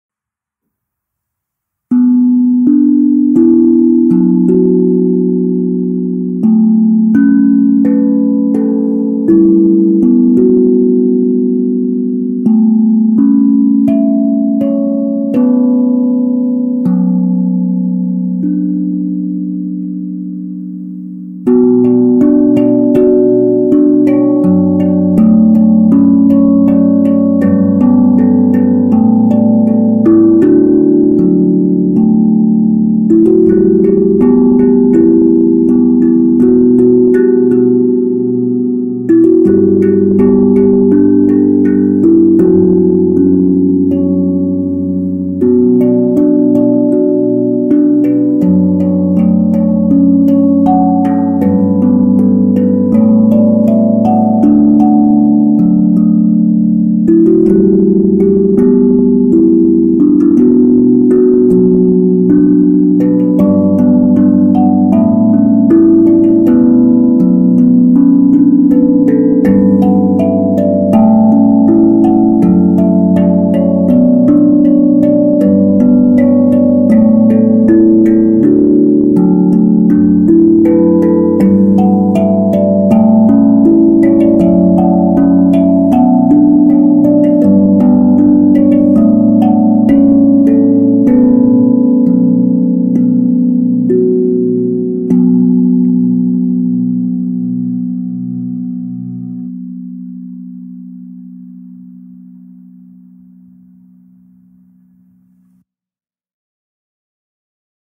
A Music of Tank Drum to Heal the Soul - Cover by April Yang
It is a new stuff (Tank Drum) this time. A beautiful melody from chinese music.

♫ MUSIC SHEETS
● Instagram ➝aprilyangxd